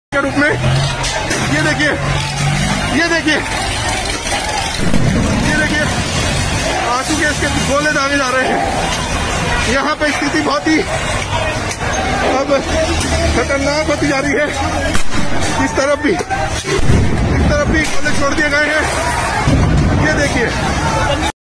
India: Video showing Delhi police shooting peaceful protesters gone viral online
如有侵权请在留言栏提醒，我会作删除处理，谢谢

india//delhi//new delhi
farmer protest//delhi police
police brutality//tactic against protestors